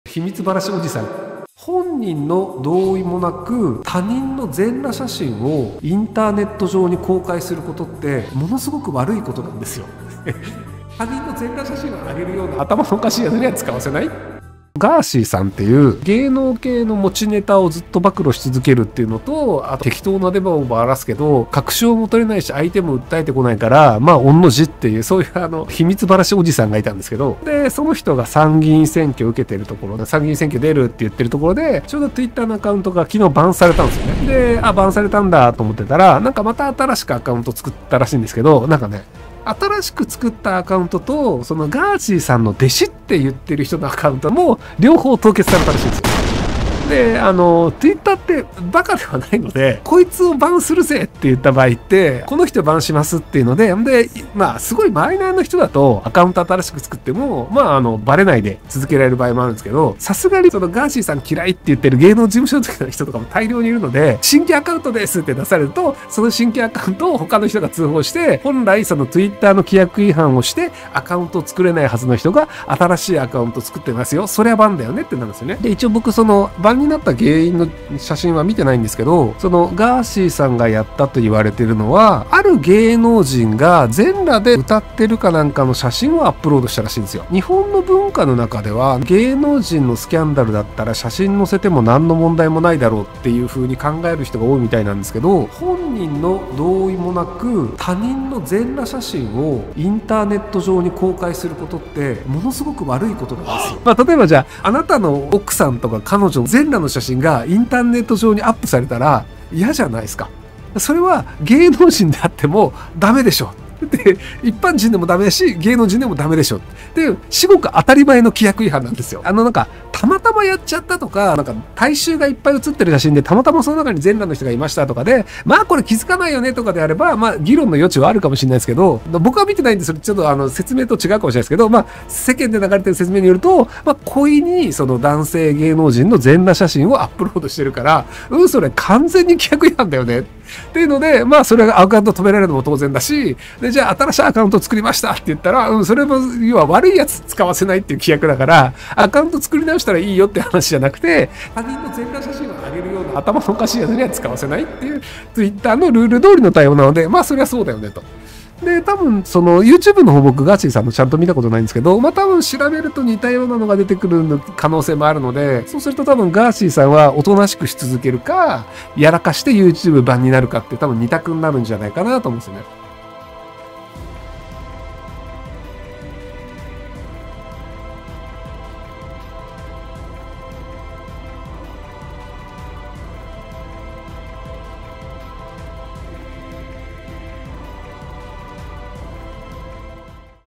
【ガーシー】小栗旬の全●を晒した秘密バラシおじさん東谷義和、twitter BAN【ひろゆき】【切り抜き】
ガーシーさんのTwitterがBANされたことについてお話ししてくれます！

ご視聴いただきありがとうございます！

🔳ホリエモン切り抜きチャンネルもやってます！